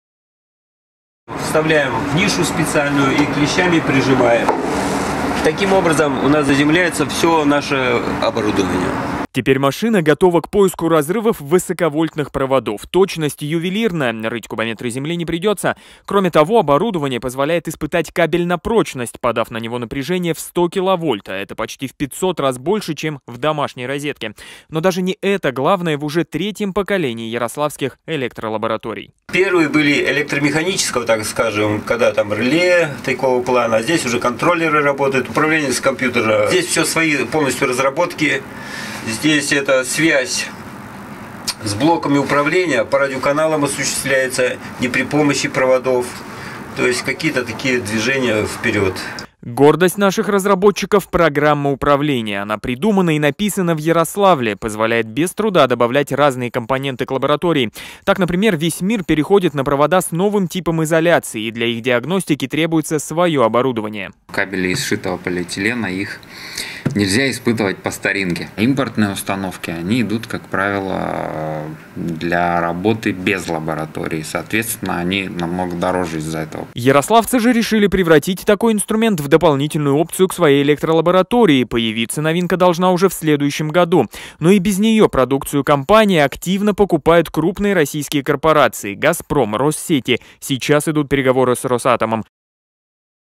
Цифровые электролаборатории «АНГСТРЕМ». Высоковольтный комплекс для испытания СНЧ-кабелей МС-62.
Разработки компании «АНГСТРЕМ» в сюжете регионального телеканала.